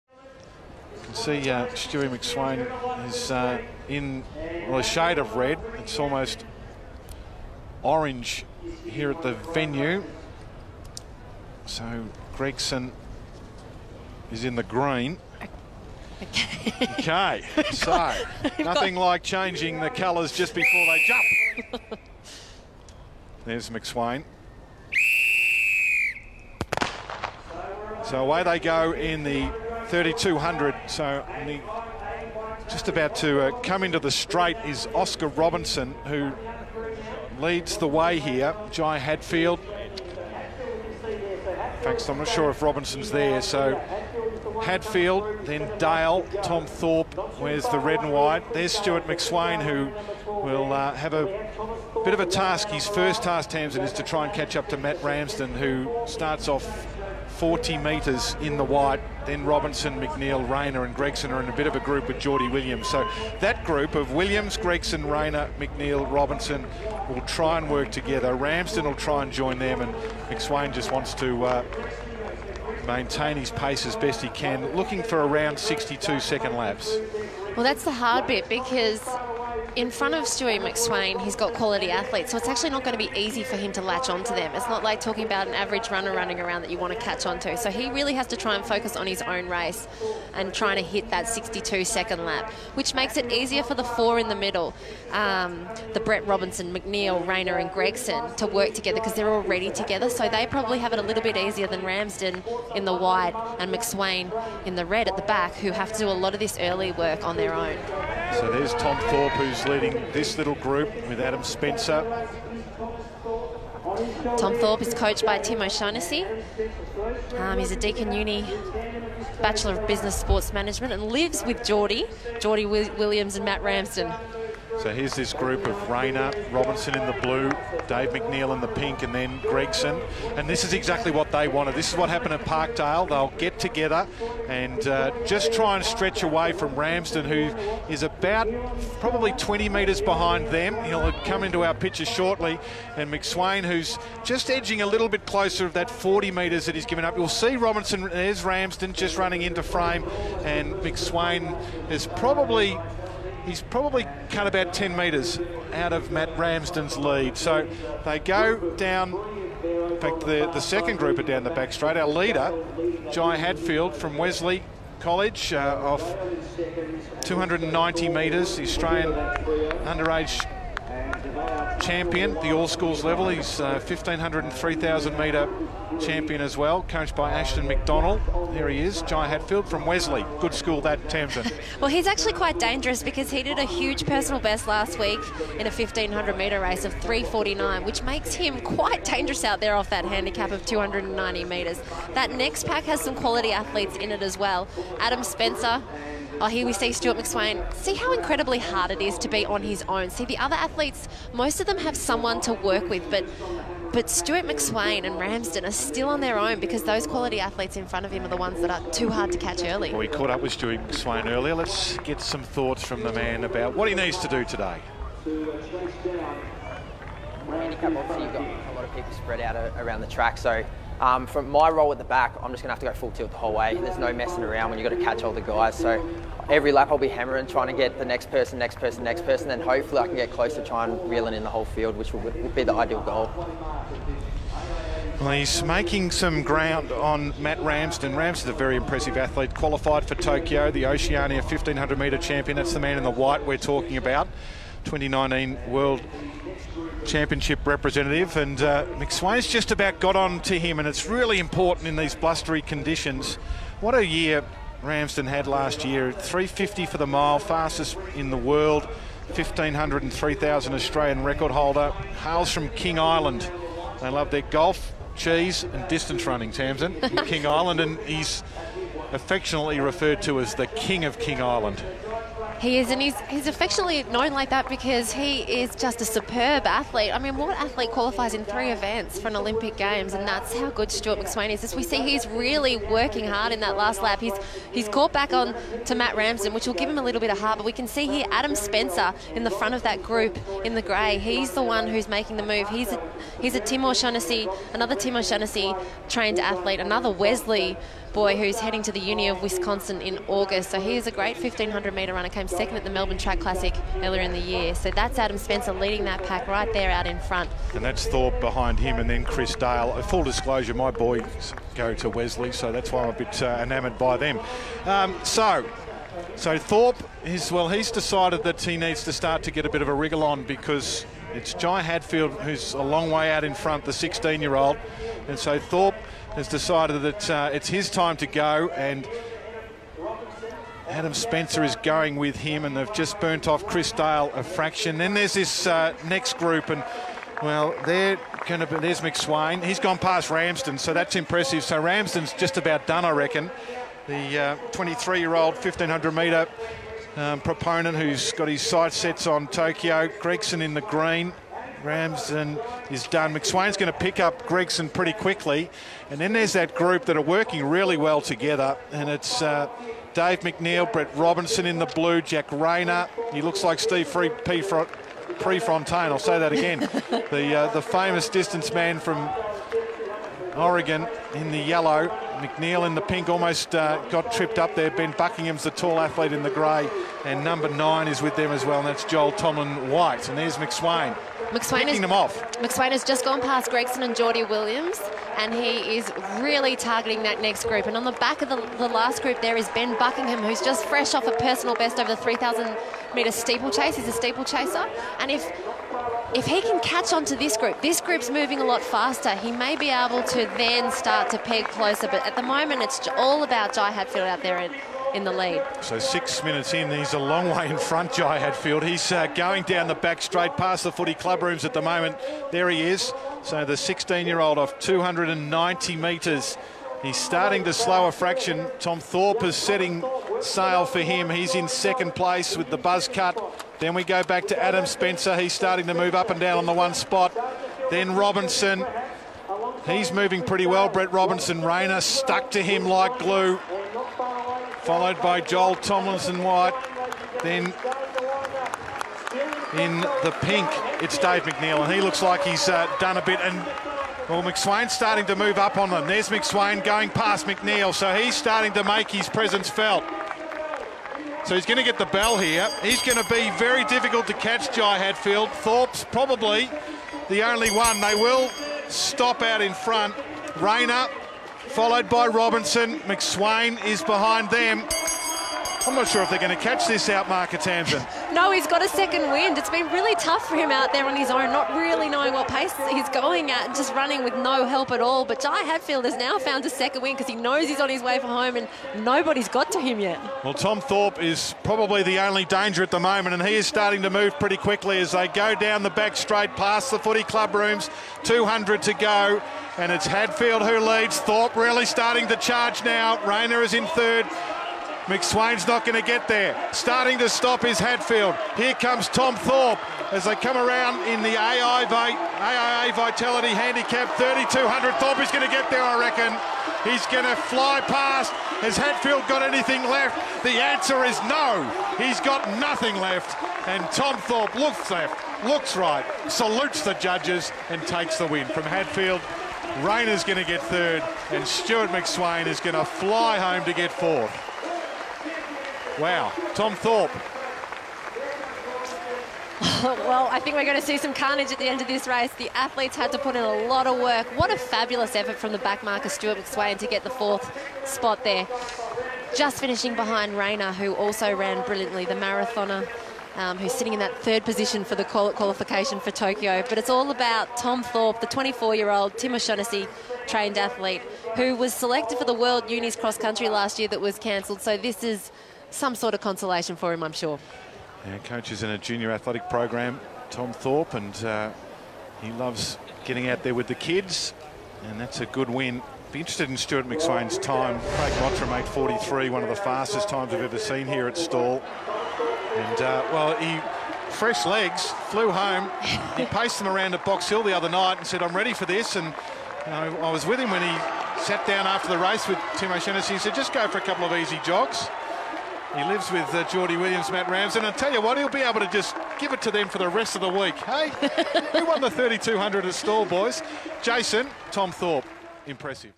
Stewart McSweyn at 3200m Handicap Race 2021
Full Race report from AIA Vitality Invitational Handicap 3200m Race at Stawell Gift carnival, Australia, April 5, 2021.
There Australian middle-distance superstar Stewart McSweyn racing off scratch in the 3200m invitational race at the Stawell Gift carnival with 12 runners ahead of him, including fellow world-class performers Ryan Gregson, Matthew Ramsden, Jack Rayner and Brett Robinson.